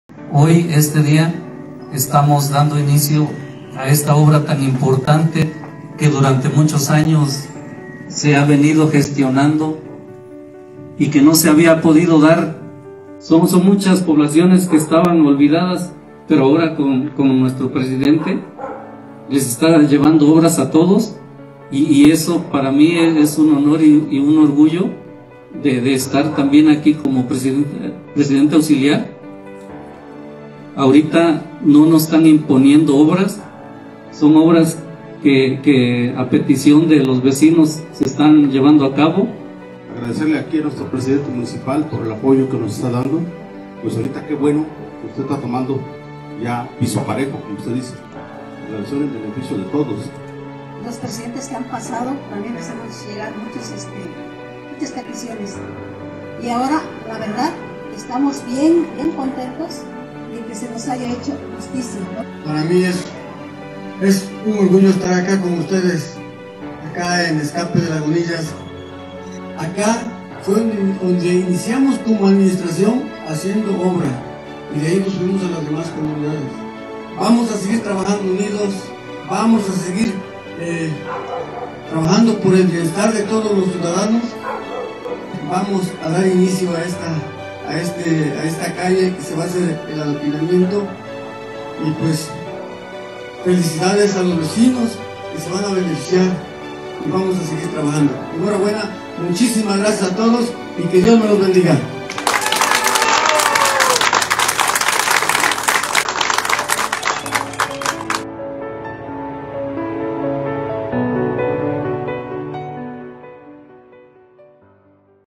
Construcción de Adoquinamiento en la calle Emiliano Zapata en Escape de Lagunillas
Mas calles dignas para los ciudadanos.

Nuestro Presidente Municipal Juan Gutiérrez Briones en compañía de la Presidenta Honorífica del DIF Municipal Chietla Minerva García Jiménez, el Regidor de Obras de Chietla, el C. Fermín Linares Calixto, el Director de obras de Chietla Óscar Paz Acevedo, y el resto de su equipo de regidores, estuvieron presentes para realizar el acto de inicio de la obra denominada “Construcción de Adoquinamiento de la calle Emiliano Zapata entre calle Benito Juárez y calle Matamoros de la comunidad de Escape de Lagunillas perteneciente al municipio de Chietla, Estado de Puebla”.

Se trata de una obra más para la dignificación de los caminos de nuestro municipio, con ello estamos mejorando la calidad de vida de las y los ciudadanos.

Chietla Ver menos